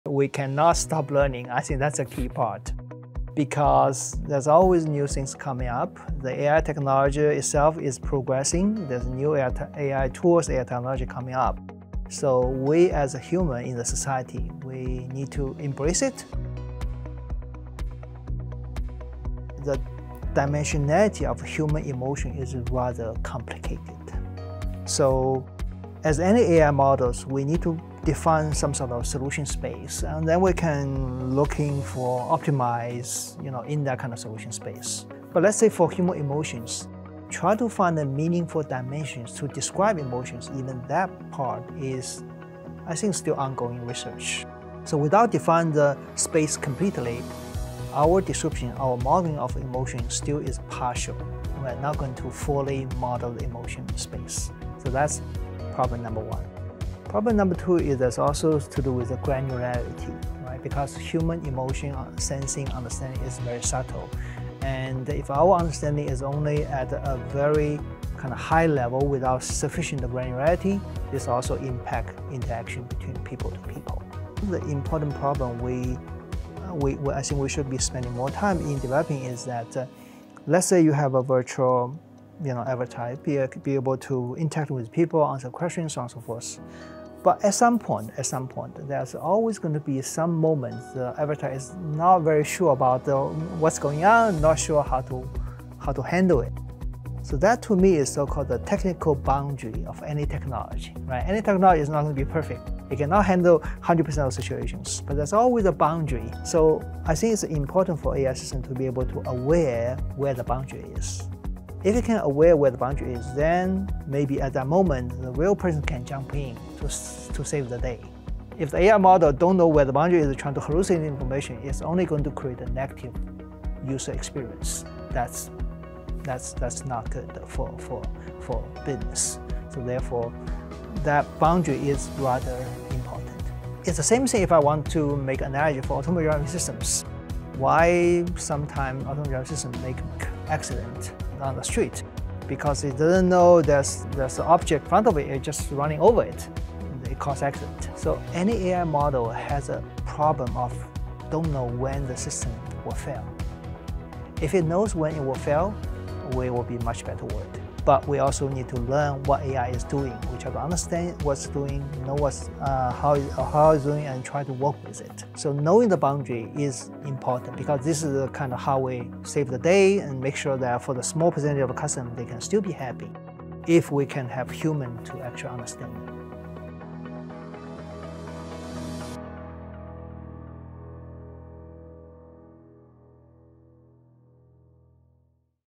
The Need for Technical Boundaries - Xiaoming Liu​
AI systems require technical boundaries that optimize human/AI collaboration and avoid creating a negative user experience.​

00:00 The AI Evolution on emotional space
01:14 AI Systems Technical Boundaries
02:28 Humans Set Technical Boundaries

Want more insights like this? Discover our 'Powered by EI, Enabled by AI' playlist TPEIandAI​